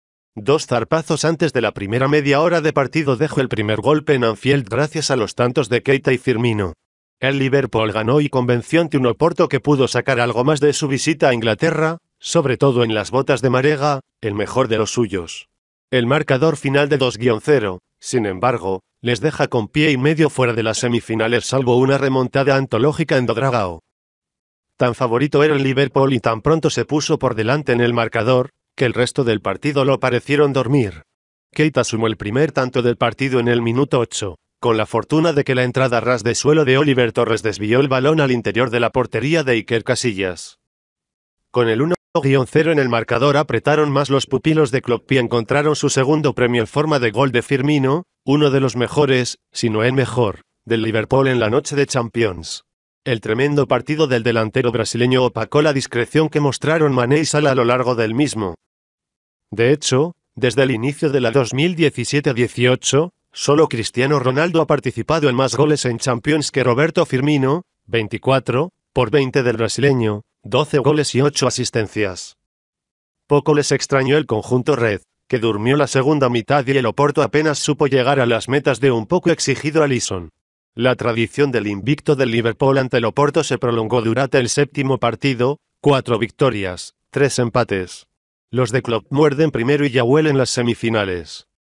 Liverpool supera con autoridad 2-0 al Porto y toma ventaja en cuartos de final de Champions League
Dos zarpazos antes de la primera media hora de partido dejo el primer golpe en Anfield gracias a los tantos de Keïta y Firmino. El Liverpool ganó y convenció ante un Oporto que pudo sacar algo más de su visita a Inglaterra, sobre todo en las botas de Marega, el mejor de los suyos. El marcador final de 2-0, sin embargo, les deja con pie y medio fuera de las semifinales salvo una remontada antológica en Do Dragao.

Tan favorito era el Liverpool y tan pronto se puso por delante en el marcador, que el resto del partido lo parecieron dormir. Keïta sumó el primer tanto del partido en el minuto 8, con la fortuna de que la entrada a ras de suelo de Óliver Torres desvió el balón al interior de la portería de Iker Casillas.

Con el 1-0 en el marcador apretaron más los pupilos de Klopp y encontraron su segundo premio en forma de gol de Firmino, uno de los mejores, si no el mejor, del Liverpool en la noche de Champions. El tremendo partido del delantero brasileño opacó la discreción que mostraron Mané y Salah a lo largo del mismo.

De hecho, desde el inicio de la 2017-18, sólo Cristiano Ronaldo ha participado en más goles en Champions que Roberto Firmino: 24, por 20 del brasileño (12 goles y 8 asistencias).

Poco les extrañó el conjunto red, que durmió la segunda mitad y el Oporto apenas supo llegar a las metas de un poco exigido Alisson. La tradición del invicto del Liverpool ante el Oporto se prolongó durate el séptimo partido: 4 victorias, 3 empates. Los de Klopp muerden primero y ya huelen las semifinales.